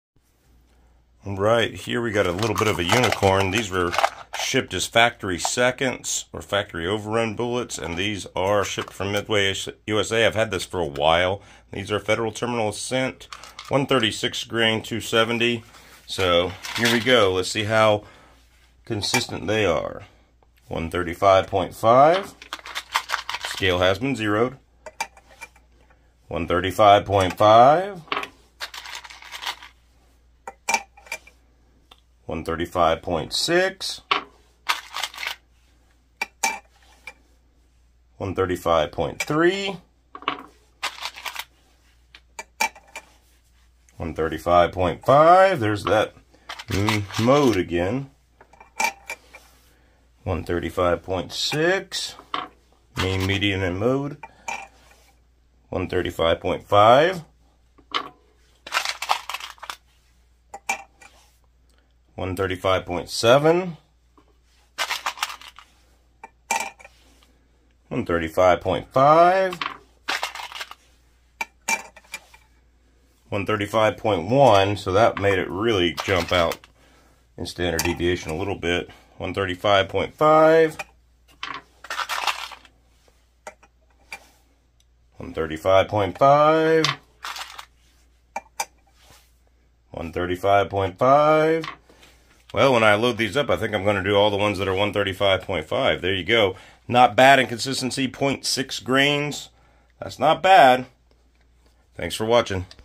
.277" 136gr Federal Terminal Ascent "Factory Seconds" weight consistency
Just weighing some factory seconds 136gr Federal Terminal Ascent .277" bullets for weight consistency.